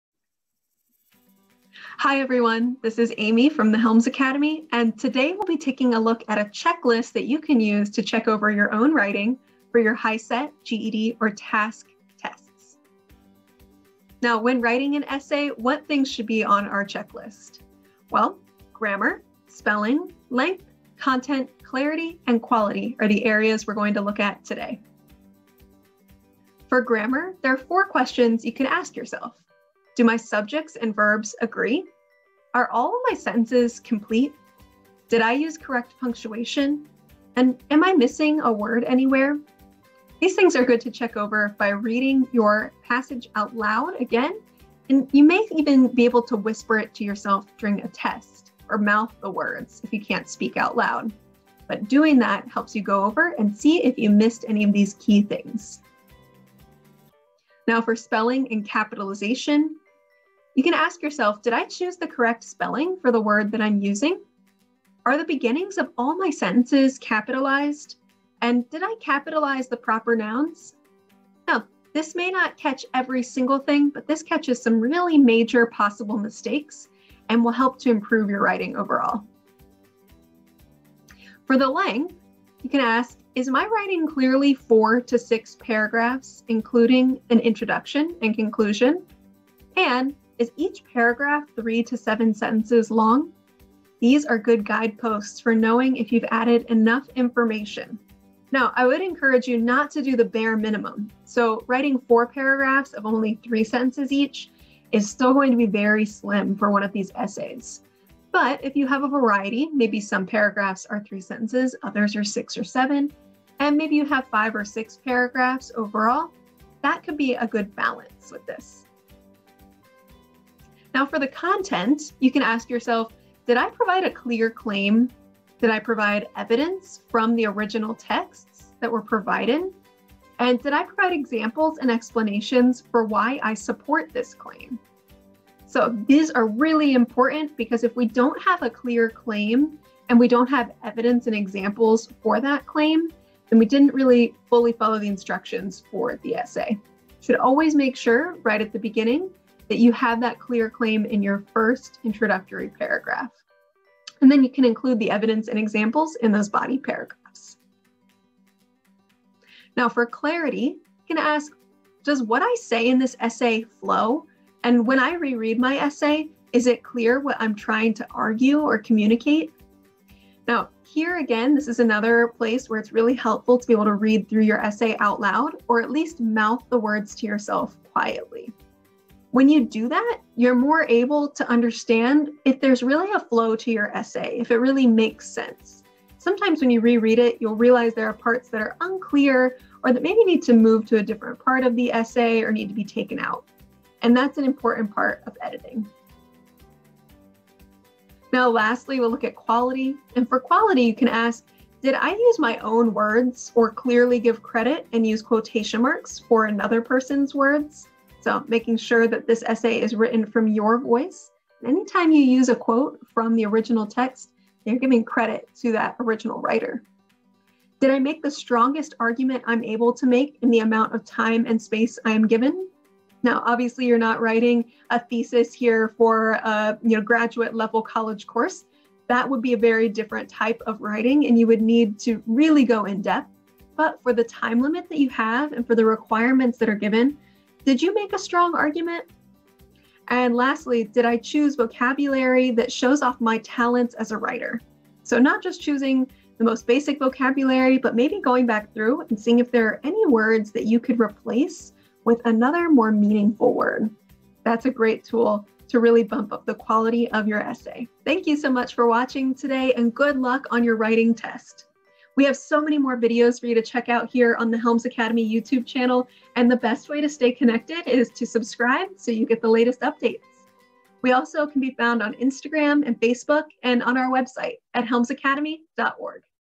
Checklist for Essay Writing | GED, HiSET, and TASC Essay Prep
Preparing for the essay portion of the test? Use this as a guide for checking your own writing!